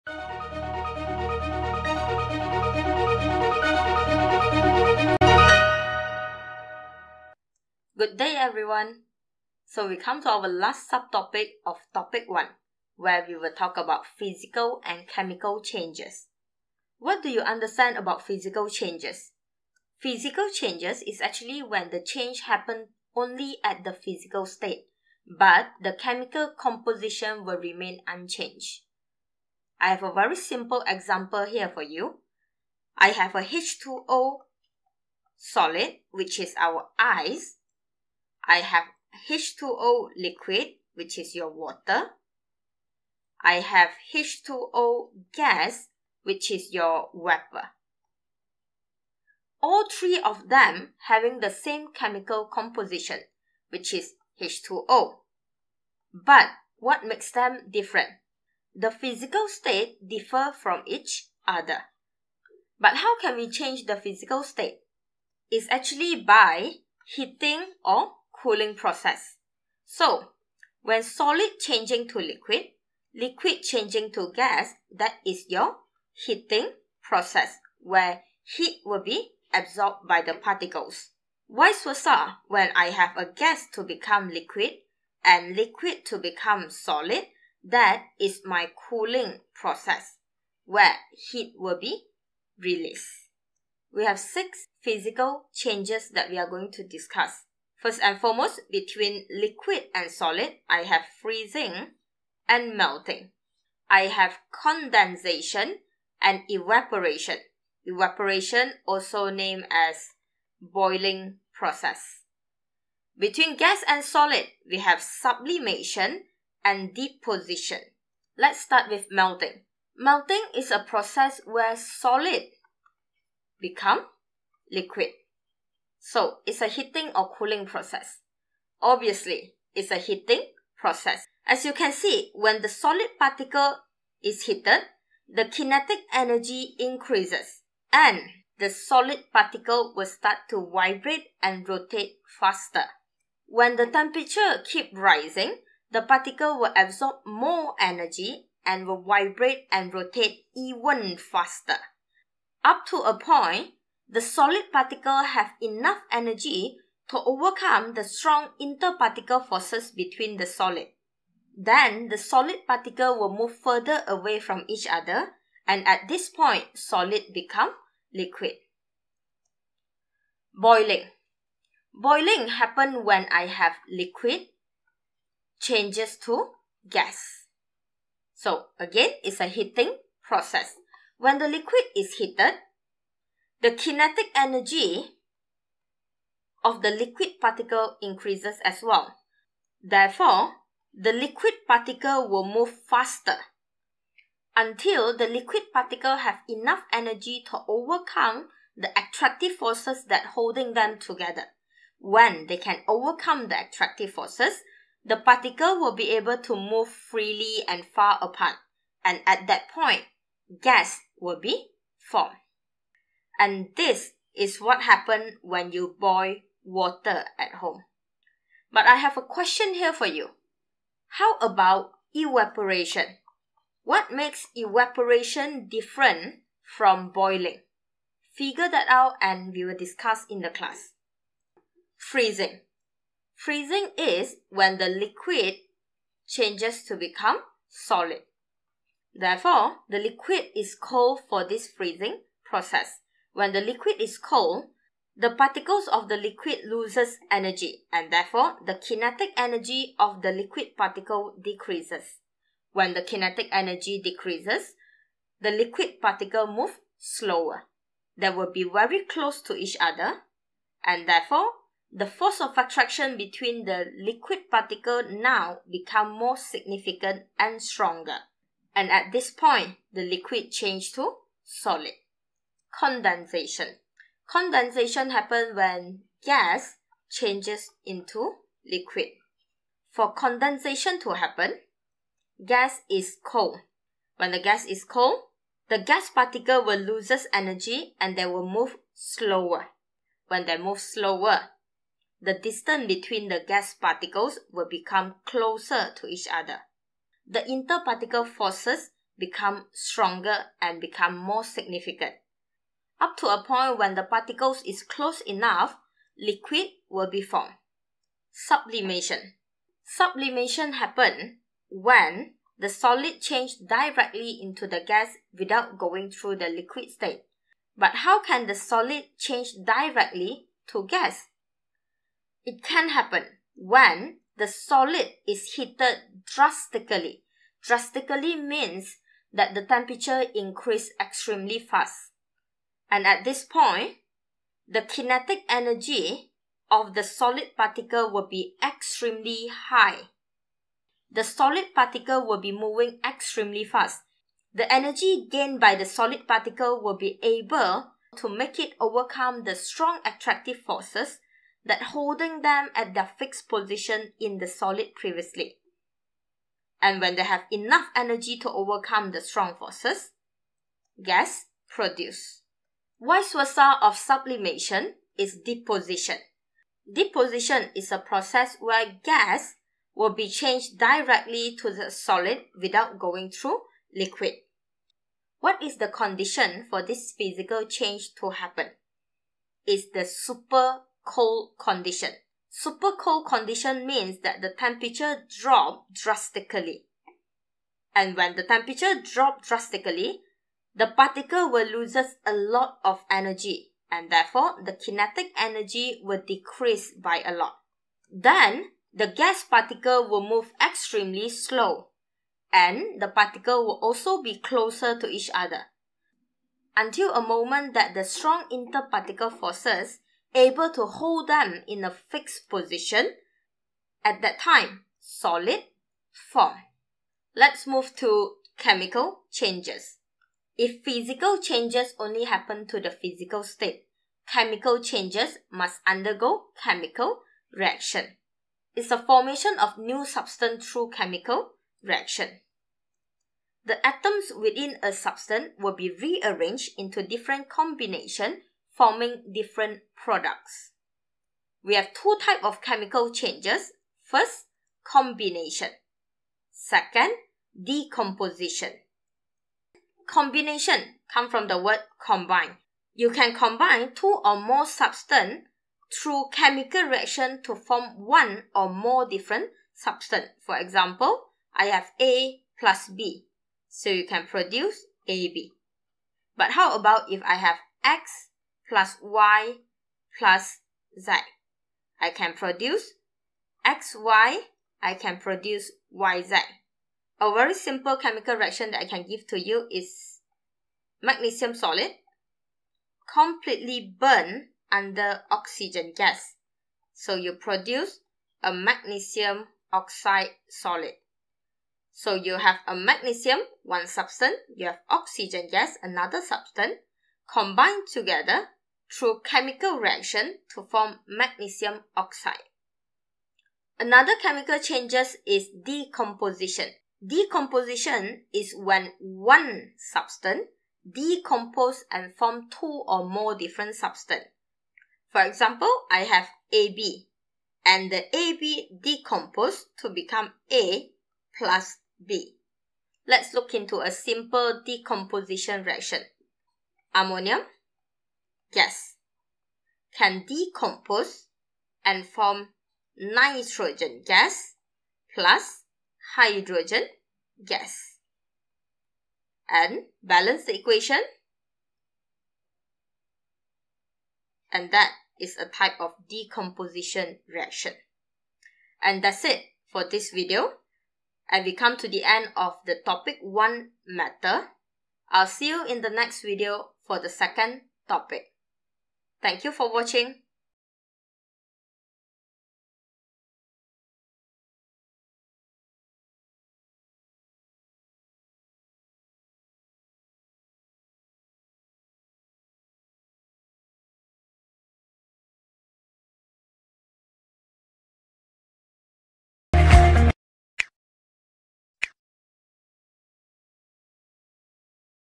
SES CHEMISTRY DK014 -- TOPIC 1.4 Physical and Chemical Changes
SES CHEMISTRY DK014 -- Topic 1.4 PHYSICAL AND CHEMICAL CHANGES [2020]

Chemistry DK014 for SES students in matriculation.

Topic 1: Matter
Subtopic 1.4: Physical and Chemical Changes
a) Describe the physical and chemical changes of matter.

Drop your comments and feedback.